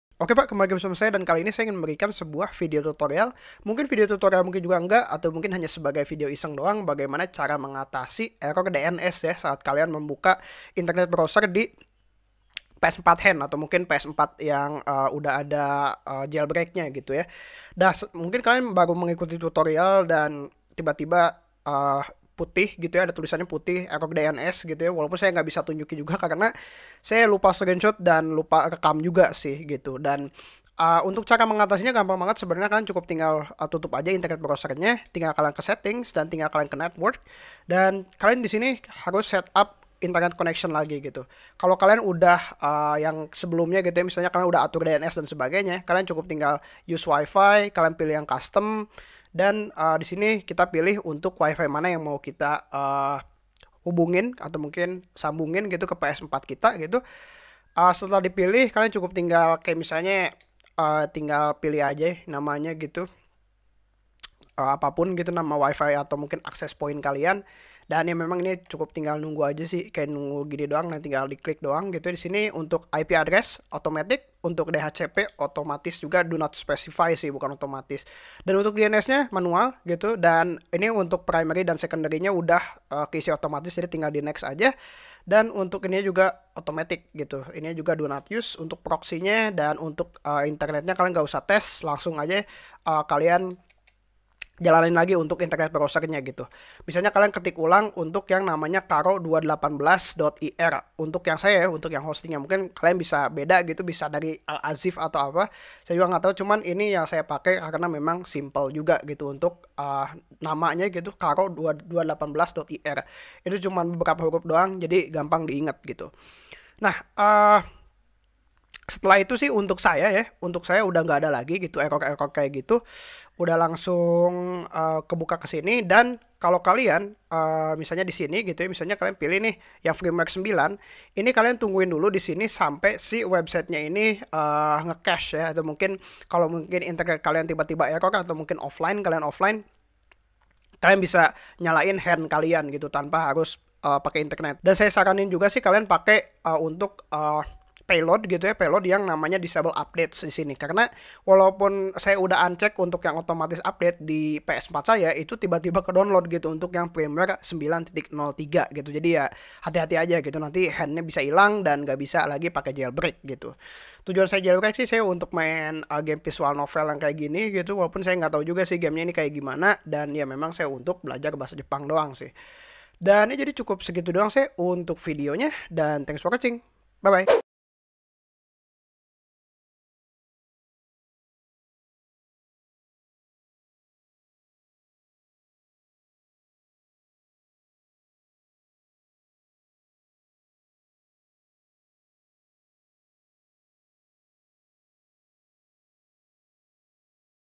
Cara Mengatasi DNS Error di PS4 Hen!
oke pak,kali ini saya akan memberikan sebuah video bagaimana cara mengatasi dns error saat membuka web host untuk jailbreak,untuk cara mengatasinya gampang banget kok.